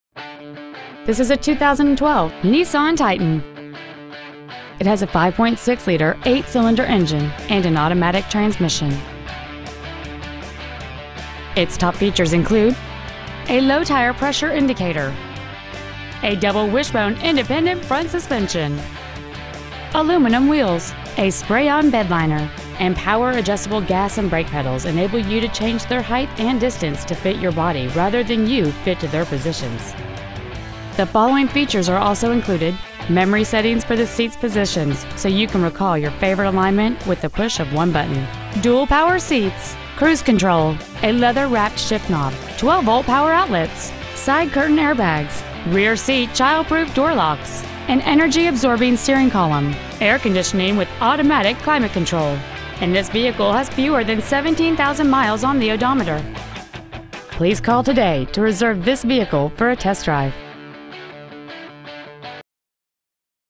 Used 2012 Nissan Titan Houston TX 77090
We have been honored to serve the Houston TX area , we promise that your experience at our dealership will exceed your expectations ! Year : 2012 Make : Nissan Model : Titan Engine : 5.6L DOHC 32-valve V8 engine Trans . : Automatic Exterior : White Miles : 16,622 Interior : Tan Stock : CN320362 Sewell Infiniti of Houston Pre-Owned North Freeway Houston , TX 77090